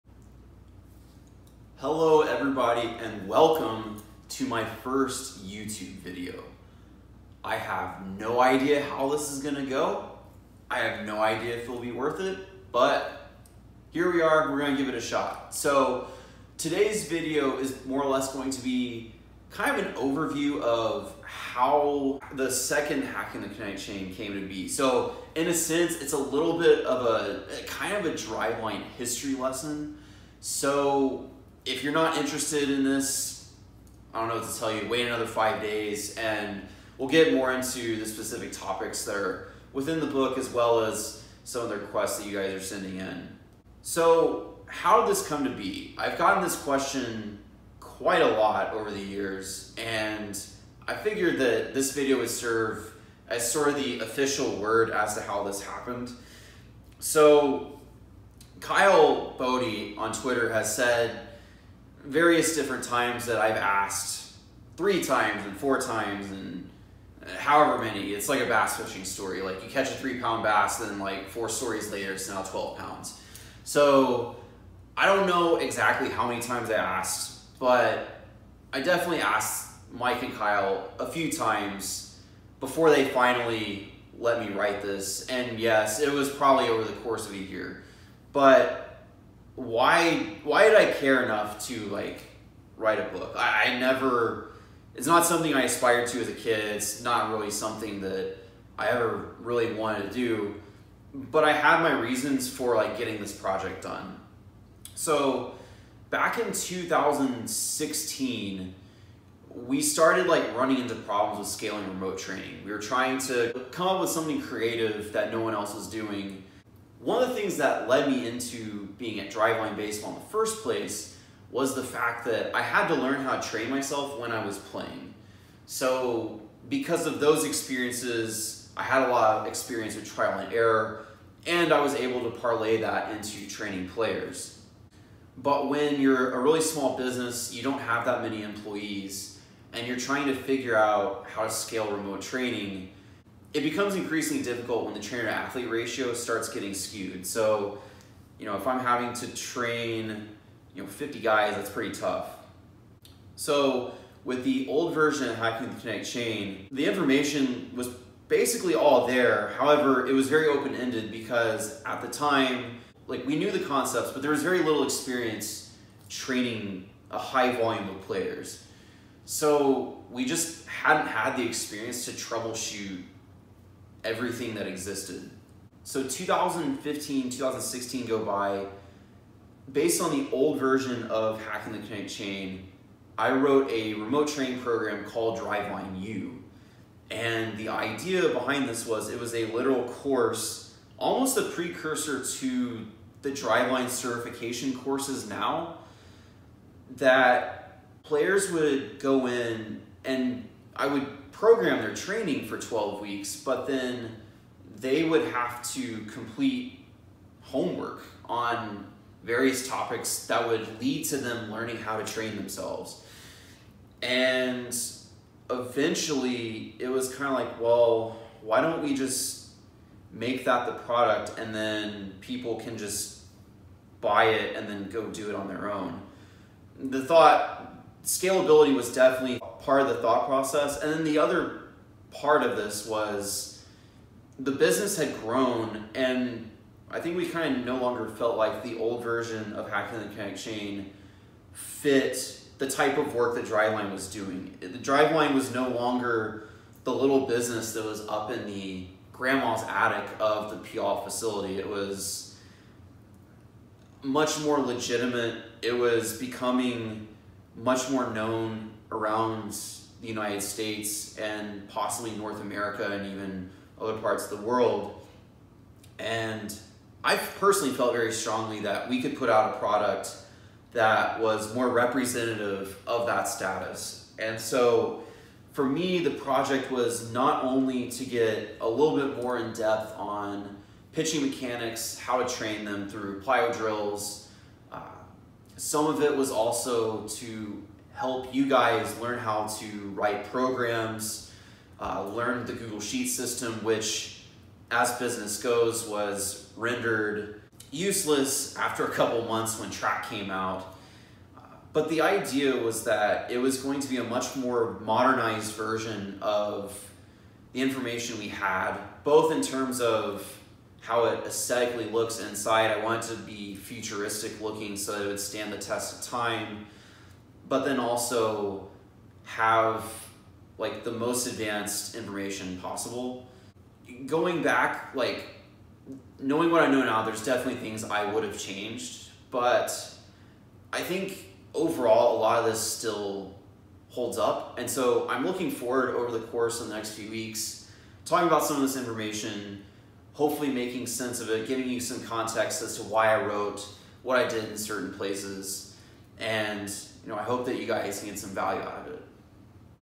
FIRST VIDEO - Why HTKC Advanced Pitching Exists
Matt gives a brief history lesson on why the second version of Hacking the Kinetic Chain exists.